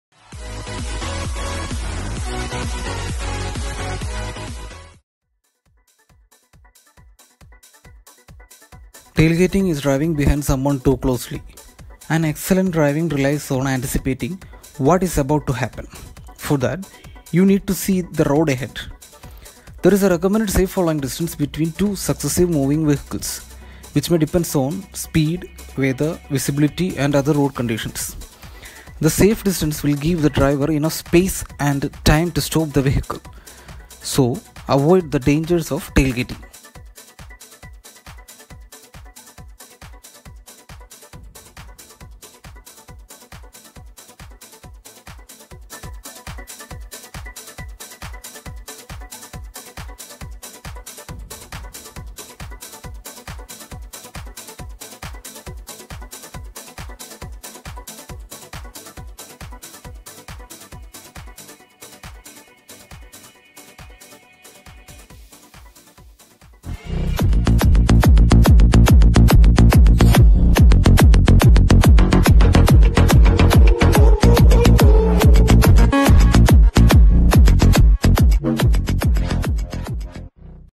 Road Safety , Tailgating
SAFER ROADS ! road safety video highlighting the dangers of tailgating.